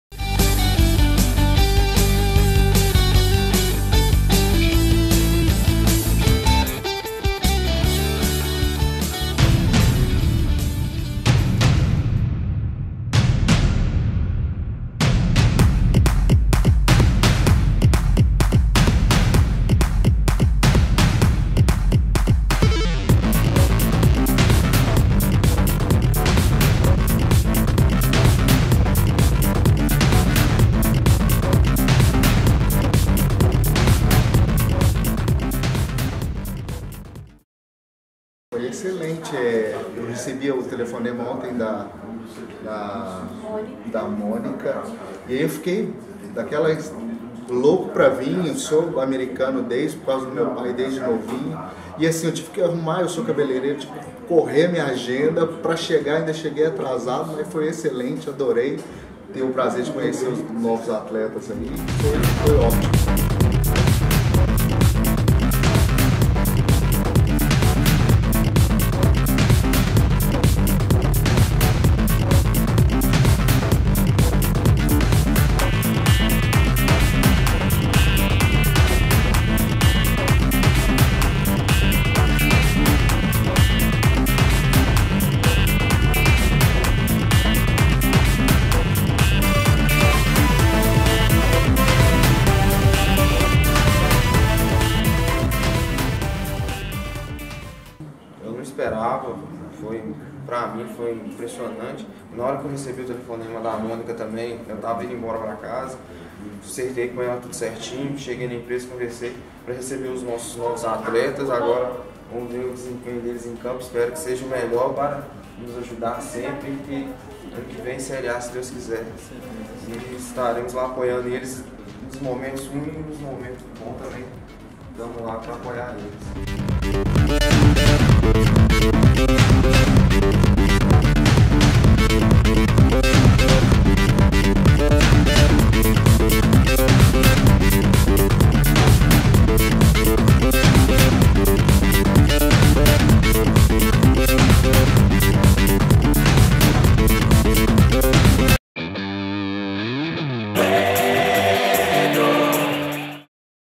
SÓCIO-TORCEDOR ONDA VERDE
América apresenta reforços com participação exclusiva de sócios-torcedores